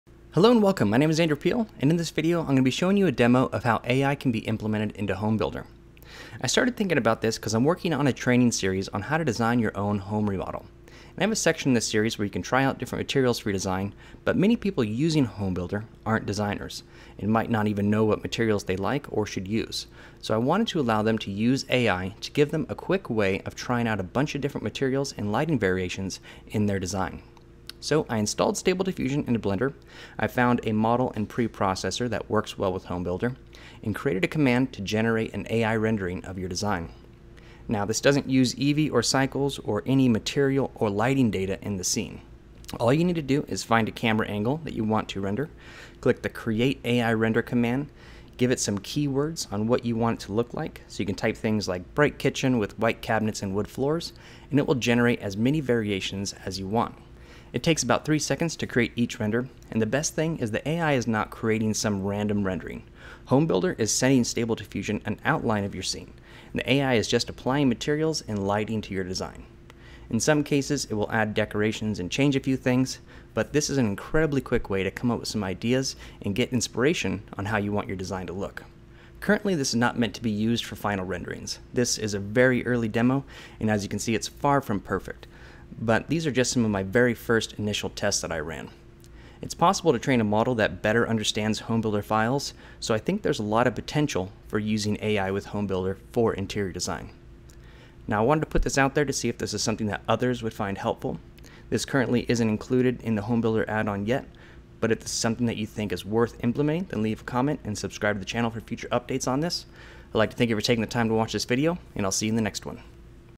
AI and Home Builder
This is a very early demo of using AI with Blender and Home Builder.